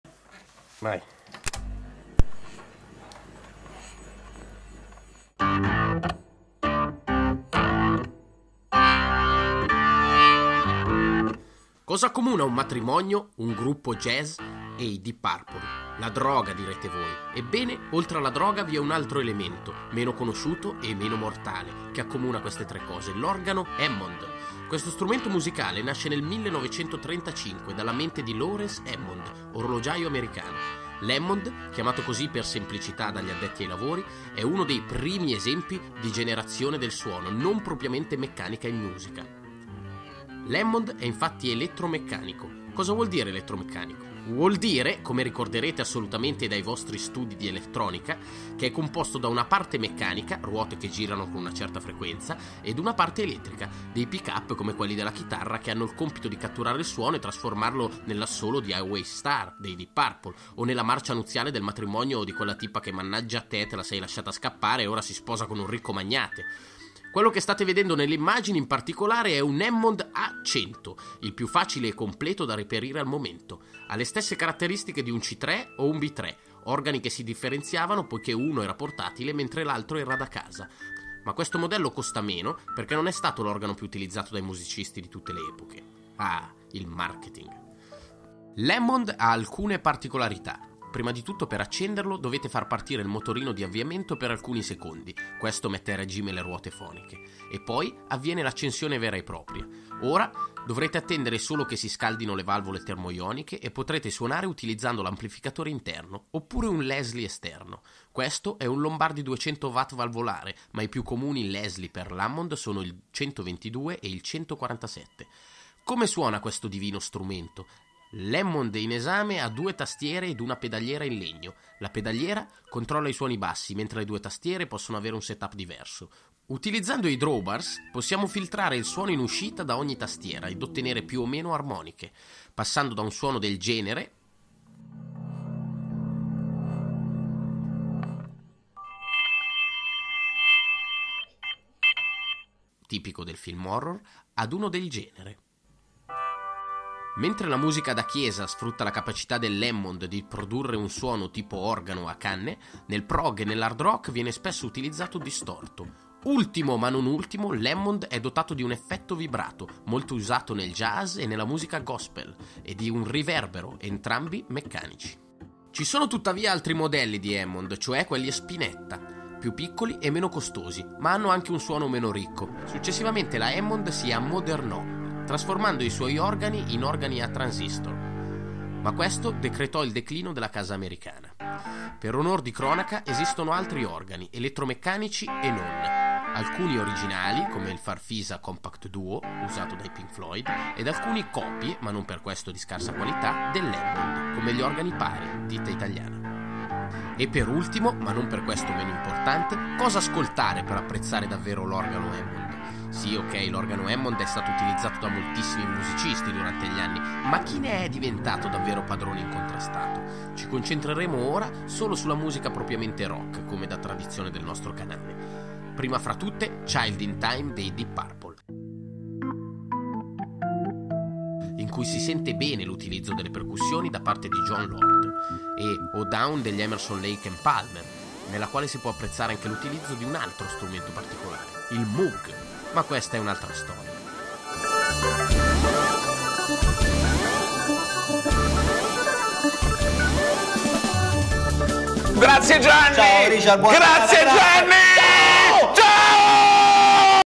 Che cos'è un Hammond? - The history of Rock
Questo video ha partecipato al concorso "2talent", e ora lo ricarichiamo.
I nostri eroi ci spiegano come funziona un fantomatico organo hammond, sogno di grandi e piccini

Il video è stato realizzato dai nostri eroi, utilizzando riprese Homemade e con la grandissima collaborazione di Matteo Jamundo alla voce/regia